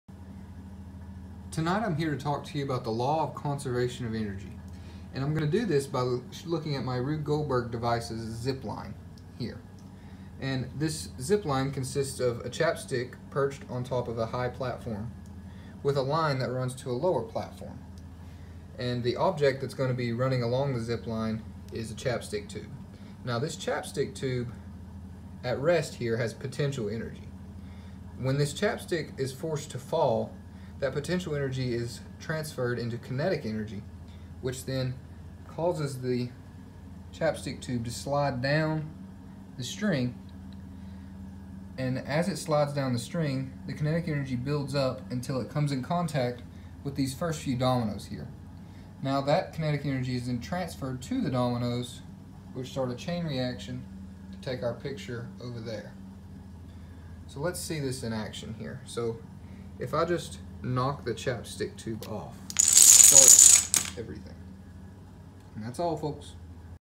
Description of Zip line
Here I talk about the Conservation if energy with the zip line portion of my RGD.